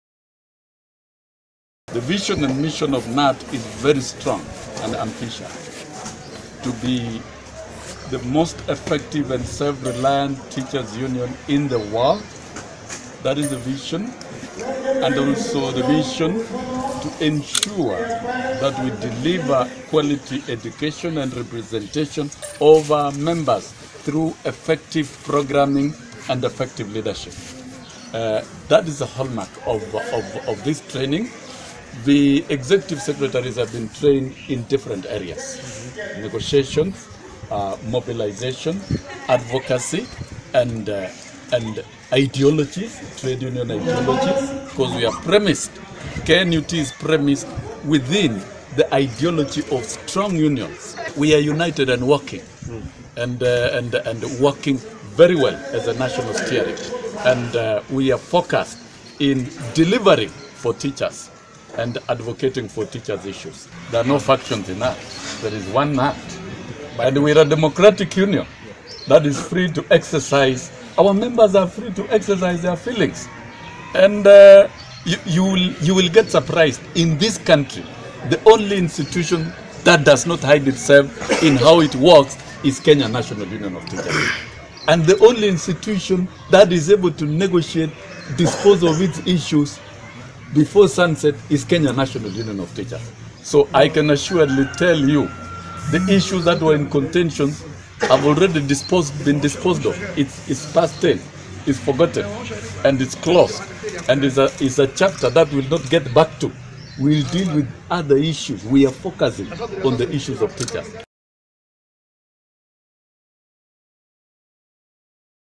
Knut chiefs claim wrangles behind them
Knut Secretary General Wilson Sossion speaks after closure of a training workshop of branch executive secretaries at Tom Mboya Labour College in Kisumu. Mr Sossion who was accompanied by other National officials said the union had agreed to bury the hatchet and forge ahead as a united front.